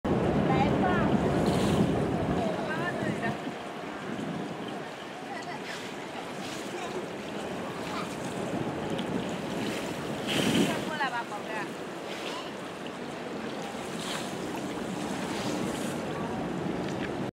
Bangkok Pool Sunset
View from the Grande Centre Point Hotel, Terminal 21.